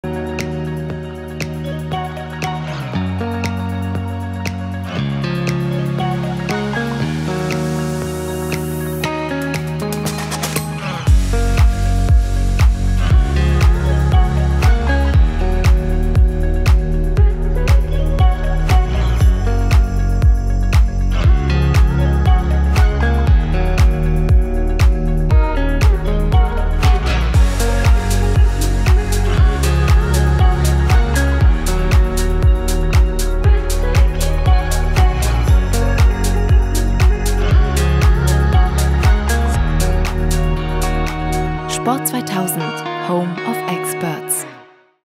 High Performance für deine nächste Tour: Der Outdoorschuh ADVENTURE TREK von WITEBLAZE 🤗🙌
High Performance für deine nächste Tour: Im Video zeigen wir dir den Outdoorschuh ADVENTURE TREK von WITEBLAZE. Features wie Echtleder, Vibram® Sohle und Made in Europe sind nur ein Auszug der Qualitätsmerkmale dieses Schuhs 🤗🙌